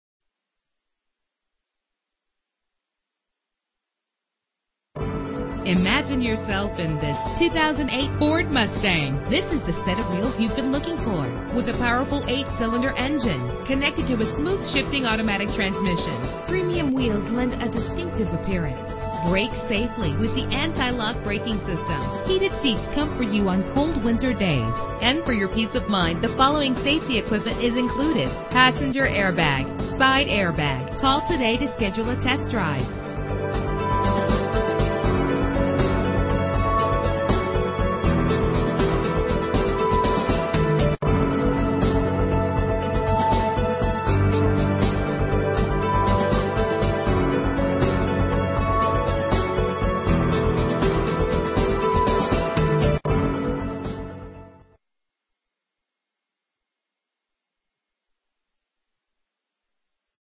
2008 Ford Mustang - Gainesville GA
Year: 2008
Make: Ford
Model: Mustang
Trim: GT
Engine: 4.6 liter 8 cylinder 24 valve
Transmission: Automatic
Color: Silver
Mileage: 28091
Address:
2801 Browns Bridge Rd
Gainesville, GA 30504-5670